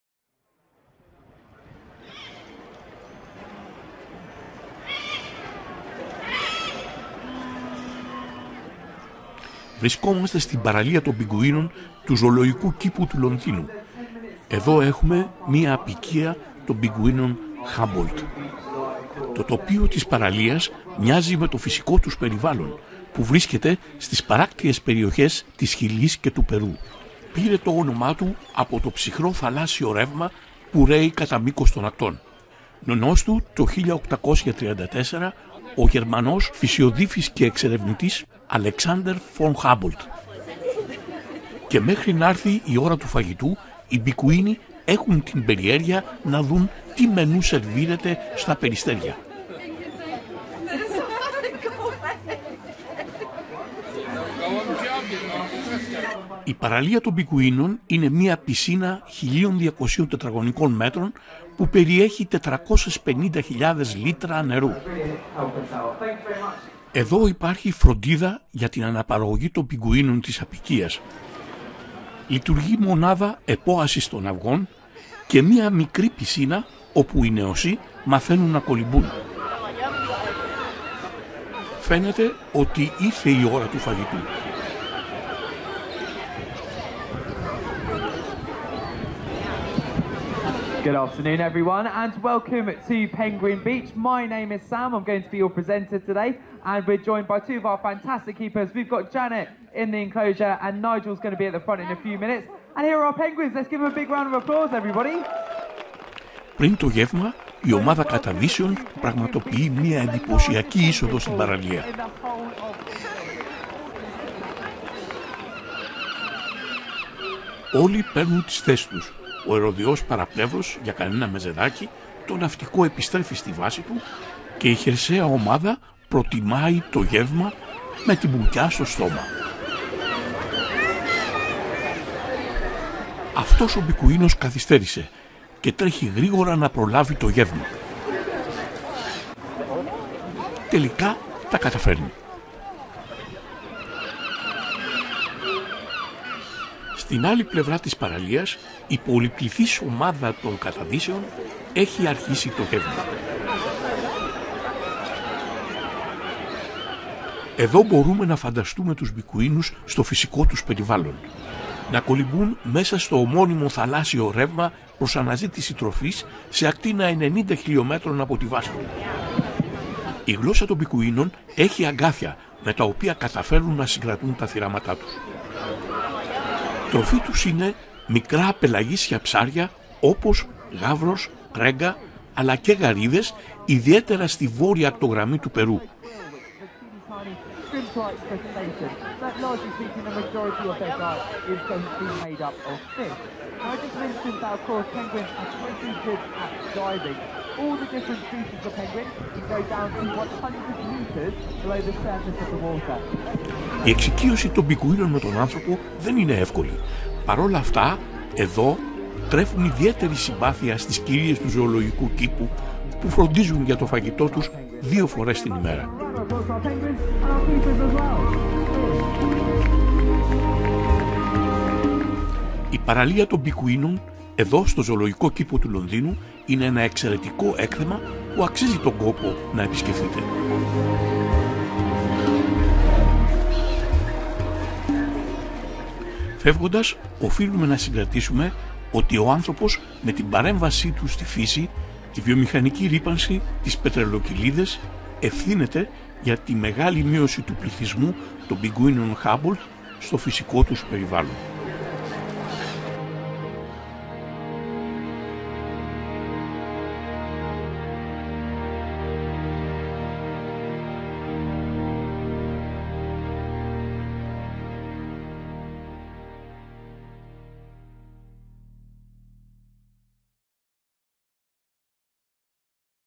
PENGUINS (Spheniscus humbolti ) London Zoo / ΠΙΓΚΟΥΙΝΟΙ ΖωολογικόςΚήποςΛονδίνου(HD) English subt
Η Παραλία των Πιγκουίνων στον Ζωολογικό κήπο του Λονδίνου είναι μία πισίνα 1200 τετραγωνικών μέτρων με 450000 λίτρα νερού, διαμορφωμένη σε τοπίο των ακτών της Χιλής και του Περου ,του βιότοπου των πιγκουίνων Humboldt
Το ψυχρό θαλάσσιο ρεύμα που ρέει σε όλο το μήκος των ακτών της Χιλής και του Περού πήρε το όνομα του από τον Γερμανό εξερευνητή και φυσιοδίφη  Alexander Von Humboldt το 1834  που το ανακάλυψε. Έδωσε επίσης το ίδιο όνομα Humboldt και στους πιγκουίνους της περιοχής  που χρησιμοποιούσαν το θαλάσσιο ρεύμα , για αναζήτηση τροφής σε ακτίνα 90 χιλιομέτρων από την βάση τους.
London Zoo Penguin Beach is 1200 square meters and contains 450000 liters of water, was a lanscape of the coasts of Chile and Peru the habitat of the Humboldt Penguins.
The cold sea current that flows along yhe coasts of Chile and Peru was named after the German explorer and naturalist Alexander Von Humboldt, who discovered it in 1834 ,gave the same name Humboldt to the penguins of the area that swam in the sea curret in search of food within a radius of 90 kilometers